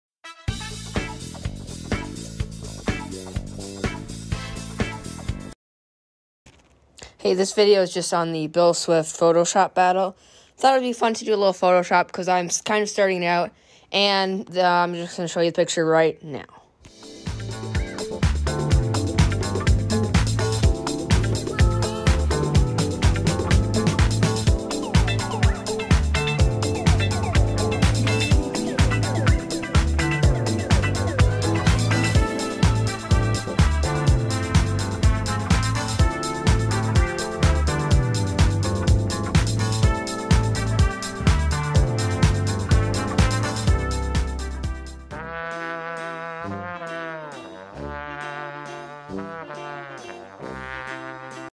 Phil Swift PhotoShop Challenge!!!
I entered the Phil Swift PhotoShop Challenge!!! And I think I did pretty well.